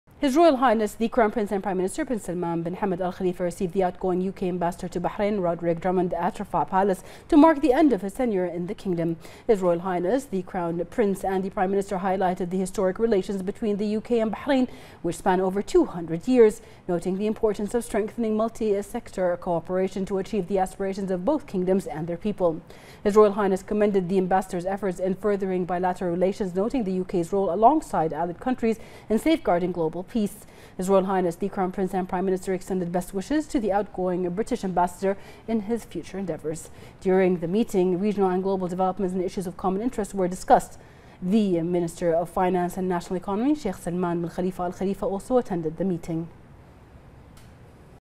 HRH the CP and PM receives the outgoing Ambassador of the United Kingdom, HE Roderick Drummond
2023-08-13

HRH the CP and PM Salman_bin_Hamad Al Khalifa receives the outgoing Ambassador of the United Kingdom to the Kingdom of Bahrain, HE Roderick Drummond